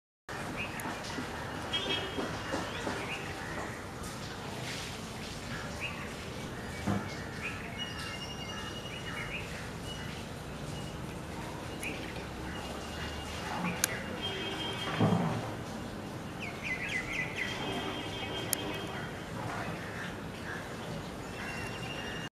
jacobin cuckoo
The Jacobin cuckoo (Clamator jacobinus), pied cuckoo or the pied crested cuckoo, is a member of the cuckoo order of birds that is found in Africa and Asia. It is partially migratory and in India, it has been considered a harbinger of the monsoon rains due to the timing of its arrival. It has been associated with a bird in Indian mythology and poetry, known as the chataka (Sanskrit: चातक) represented as a bird with a beak on its head that waits for rains to quench its thirst.